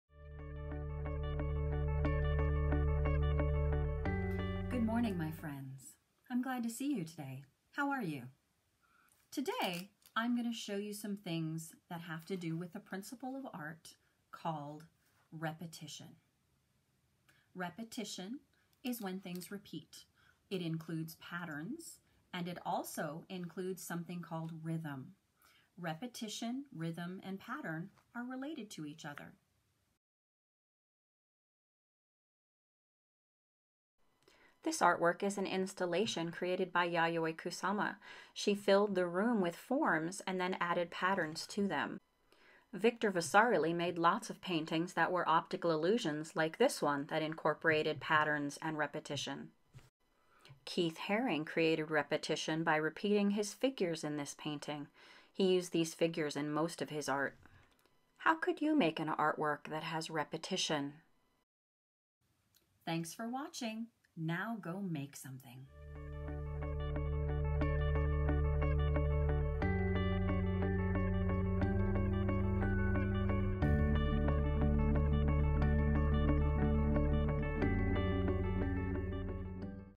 Principles of Art: Repetition
How have some artists used the principle of art called Repetition?